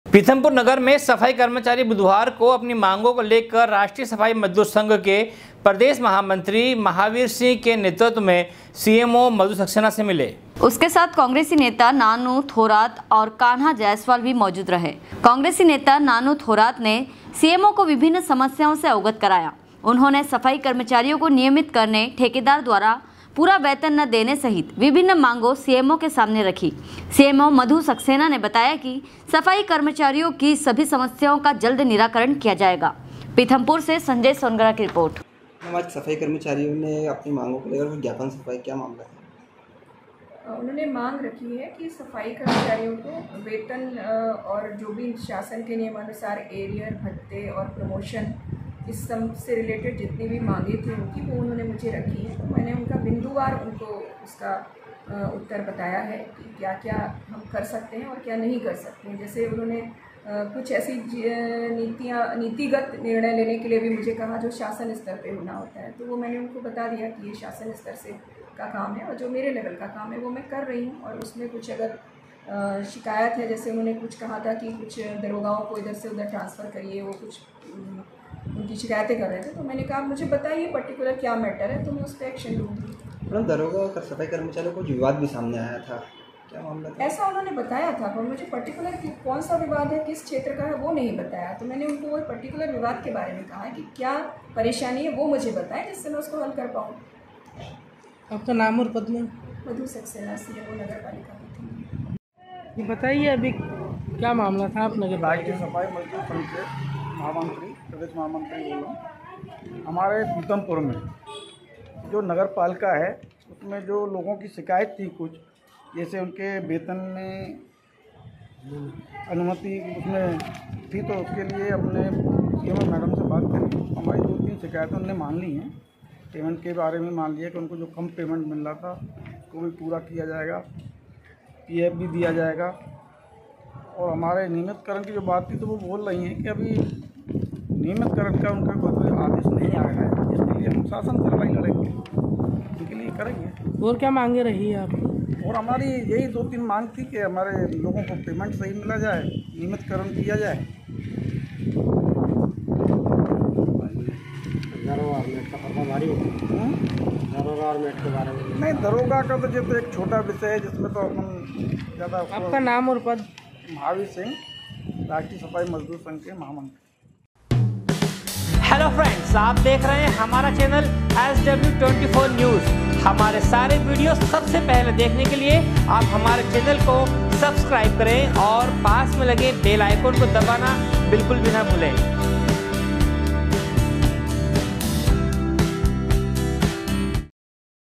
पीथमपुर नगर के सफाई कर्मचारी बुधवार को अपनी मांगों को लेकर सीएमओ मधु सक्सेना से मिले।
पीथमपुर नगर के सफाई कर्मचारी बुधवार को अपनी मांगों को लेकर राष्ट्रीय सफाई मजदूर संघ के प्रदेश महामंत्री महावीर सिंह के नेतत्व मे सीएमओ मधु सक्सेना से मिले।
पीथमपुर से संजय सोनगरा रिपोर्ट